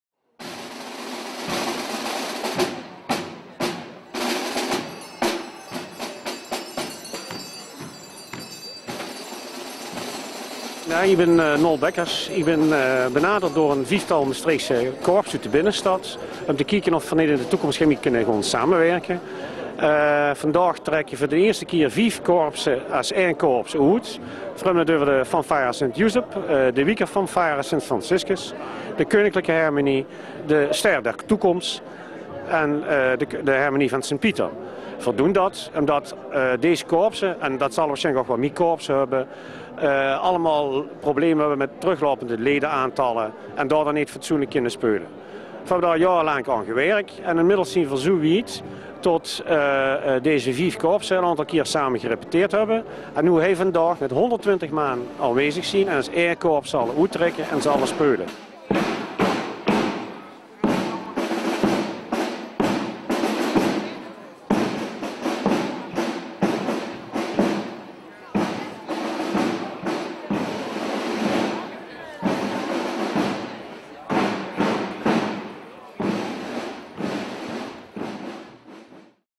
5 Samenwerkende muziekkorpsen - Stadsprocessie 2010
De 5 samenwerkende muziekkorpsen uit Maastricht tijdens Stadsprocessie 2010.

(origineel gepubliceerd op 21 mei 2010)